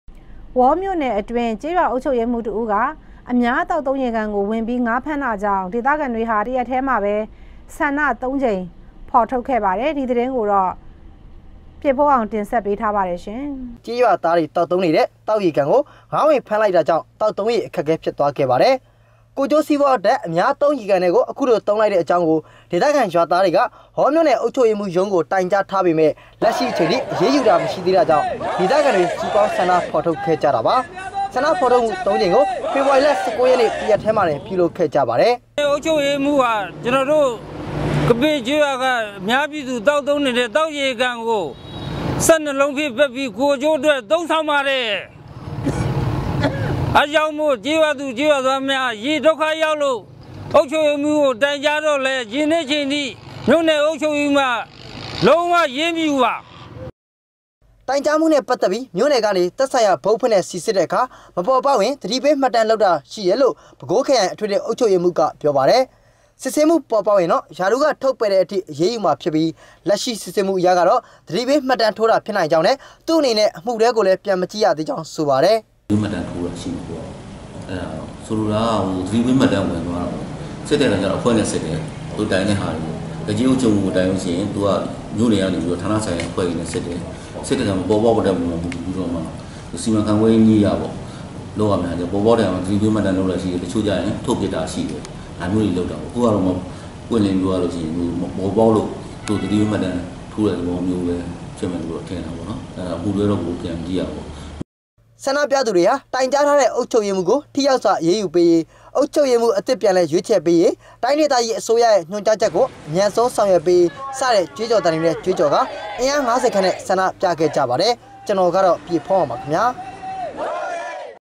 DVB - တစ္ရက္ထဲမွာတင္ ဆႏၵ ၃ ႀကိမ္ထုတ္ေဖာ္
ေဝါၿမိဳ႕နယ္ အတြင္း ေက်းရြာအုပ္ခ်ဳပ္ေရးမွဴးတစ္ဦးက အမ်ားေသာက္သံုးေရကန္ကို ငါ၀င္ဖမ္းတာေျကာင့္ ေဒသခံေတြဟာ တစ္ရက္ထဲမွာတင္ ဆႏၵ ၃ ႀကိမ္ထုတ္ေဖာ္ခဲ့ပါတယ္။ ဒီသတင္းကိုေတာ့ ျပည့္ဘုန္းေအာင္က တင္ဆက္ေပးမွာပါ။ ရိုက္ကူး-ေအာင္စိုးမိုး